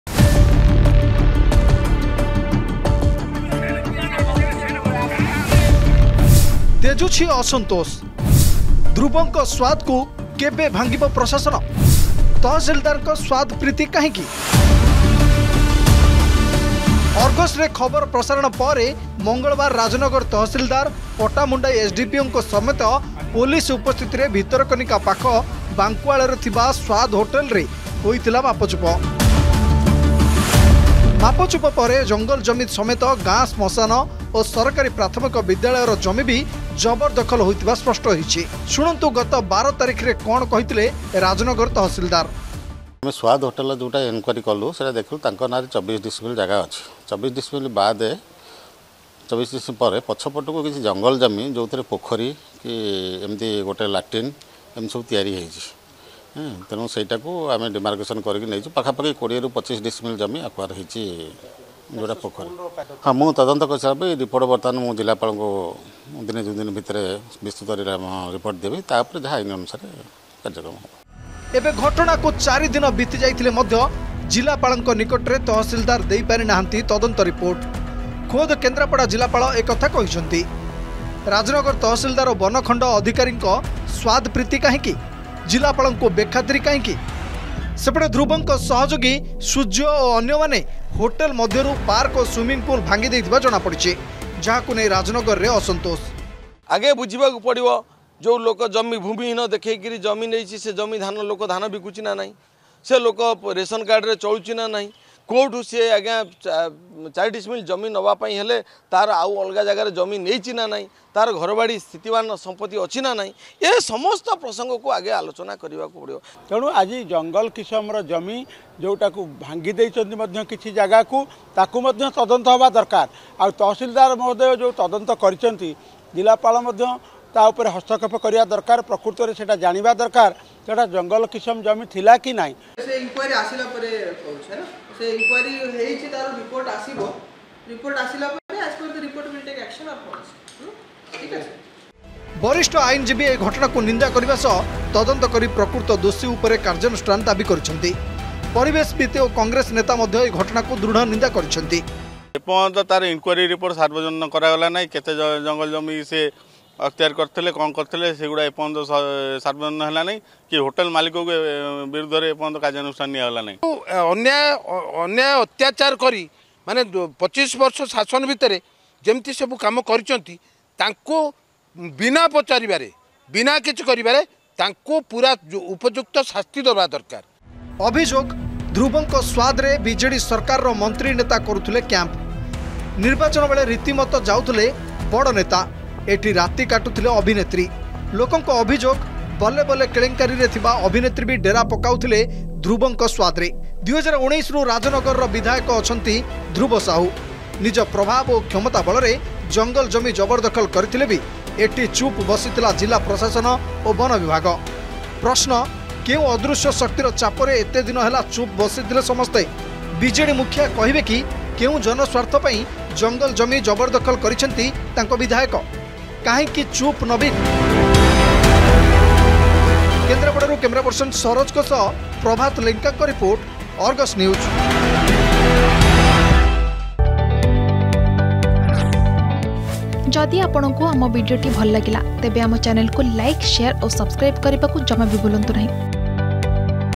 ହୋଟେଲ ସ୍ବାଦକୁ ନେଇ ରାଜନଗରରେ ତେଜୁଛି ଅସନ୍ତୋଷ || Swad Hotel
ହୋଟେଲ ସ୍ବାଦକୁ ନେଇ ରାଜନଗରରେ ତେଜୁଛି ଅସନ୍ତୋଷ । ଧ୍ରୁବଙ୍କ ସ୍ବାଦକୁ କେବେ ଭାଙ୍ଗିବ ପ୍ରଶାସନ ? ତହସିଲଦାରଙ୍କ ସ୍ବାଦ ପ୍ରିତି କାହିଁକି ? କାହିଁକି ଜିଲ୍ଲାପାଳଙ୍କୁ ତଦନ୍ତ ରିପୋର୍ଟ ଦେଉନାହାନ୍ତି ତହସିଲଦାର । ଏପରି ଅନେକ ବଡ଼ ପ୍ରଶ୍ନ ସାମ୍ନାକୁ ଆସିଛି ।